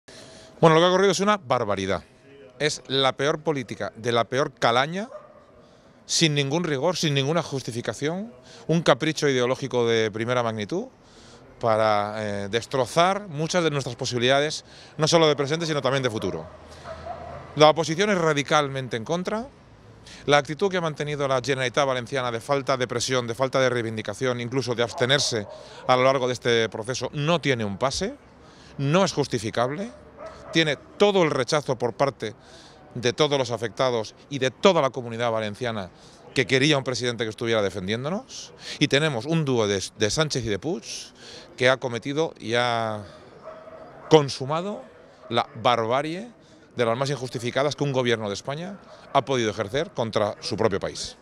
Mazón tilda de “capricho ideológico” el recorte del trasvase y culpa a Puig y Sánchez
El líder del PPCV augura que esta decisión va a destrozar el futuro y el presente de miles de familias y cree que la decisión es “una barbaridad, la peor política, de la peor calaña”.